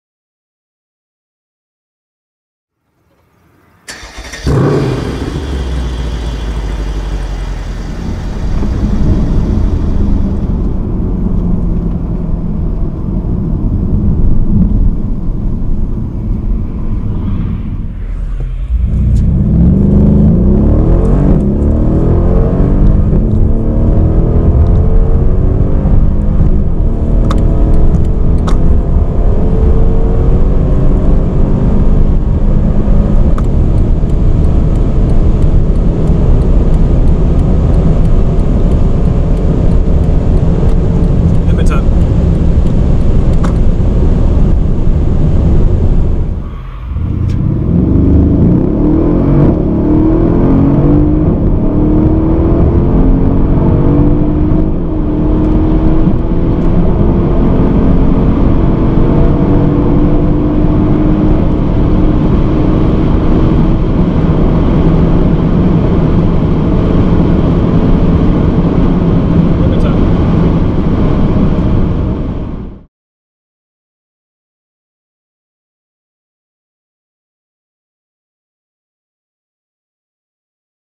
2014 Mercedes CLS 63 AMG (558hp) - 0-253 km/h acceleration (1080p)
Super fast Autobahn acceleration test with the 2014 Mercedes-Benz CLS 63 AMG powered by a 5.5 liter V8 Biturbo that delivers 558 hp and 720 Nm of torque. This particular car comes with the optional 4Matic AWD system that I would recommend everybody since the traction is superb, mated by the very quick shifting AMG Speedshift transmission. This therefore is the quickest 0-100 km/h time that I have ever tested although that the speedometer seems to be very accurate (250 speedo = 250 limiter). The sound also is typical AMG despite the fact that two turbos steal some exhaust air. Please watch out for more videos of this car as I will be uploading the full DRIVE & SOUND very soon.